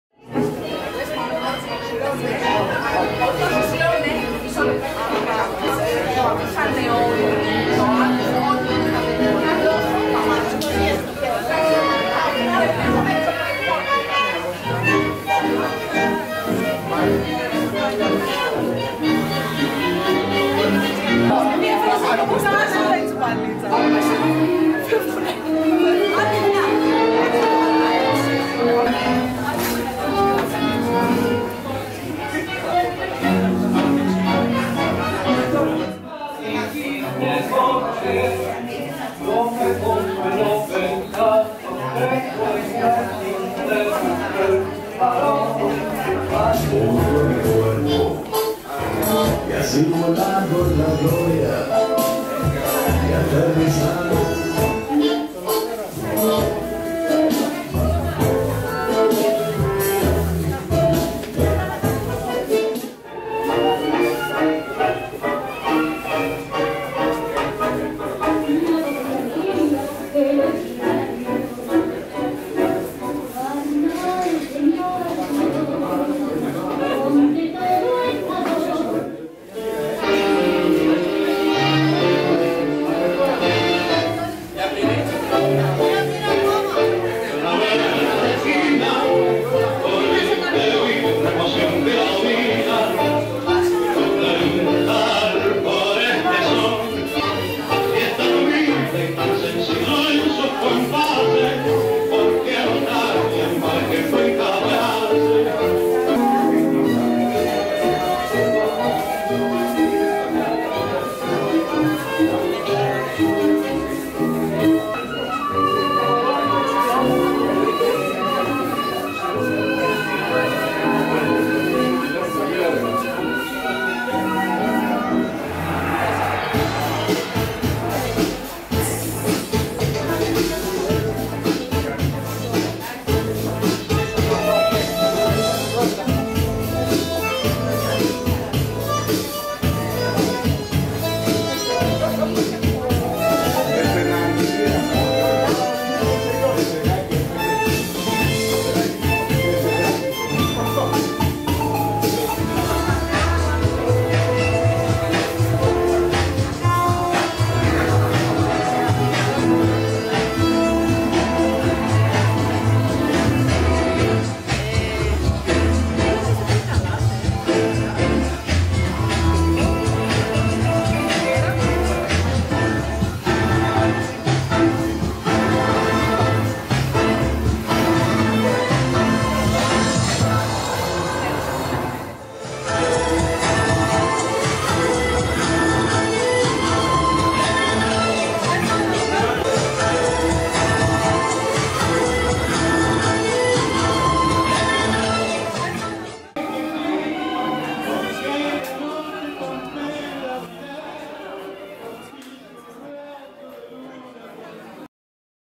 ALEGRIA - TJ POLYTIMH - JULY 2019 3'
TANGO κυριακάτικες βραδιές με ALEGRIA ... χορευτική εγρήγορση και μουσική απογείωση των Μεγάλων Μαέστρων...